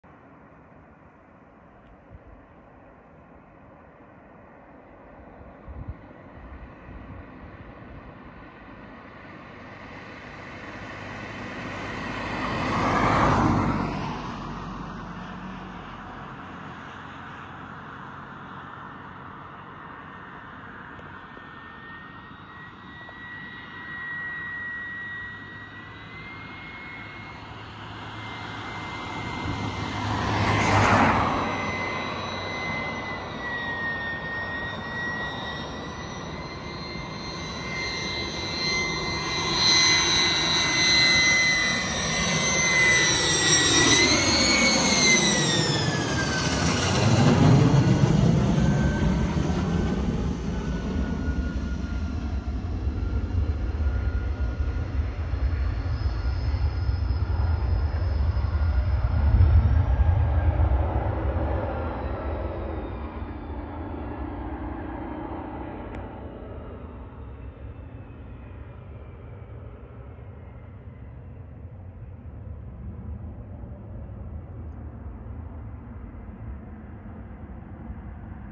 U2 (Black 01) Landing at RAF Fairford UK
Black 01 returning to RAF Fairford October 2022